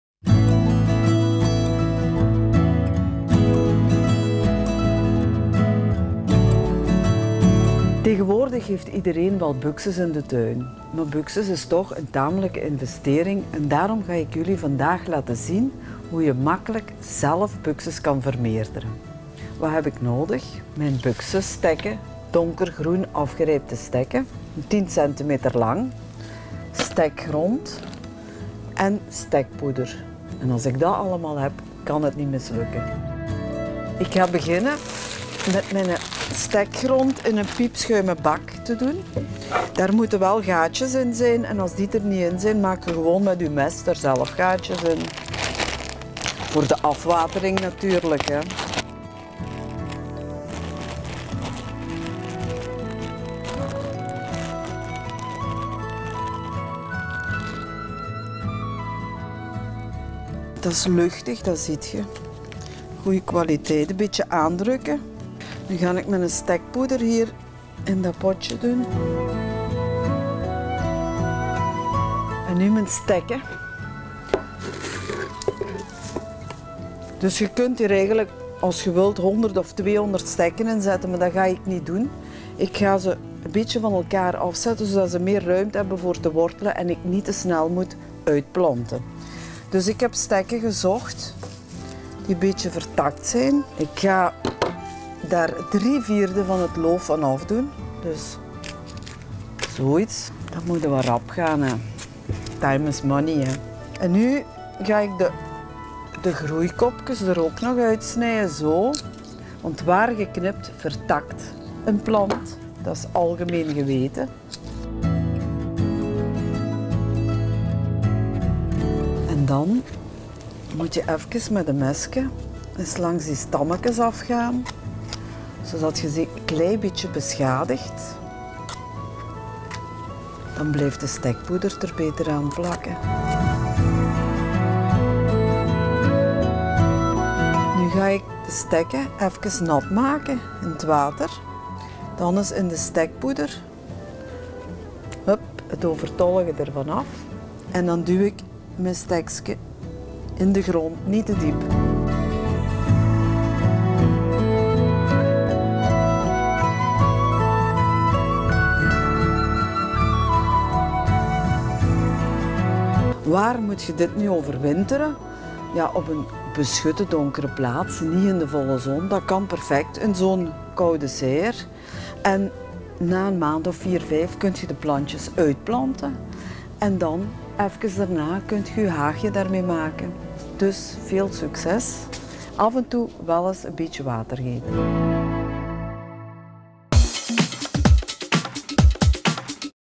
TUIN: hoe kan ik buxus vermeerderen
TJING TJING - Iedereen heeft tegenwoordig wel wat buxus in de tuin. Wie graag nog wat meer buxus in de tuin wil, kan dat zelf doen. Dina Deferme toont ons hoe je makkelijk de buxus in je tuin zelf kan vermeerderen. [29.09.12]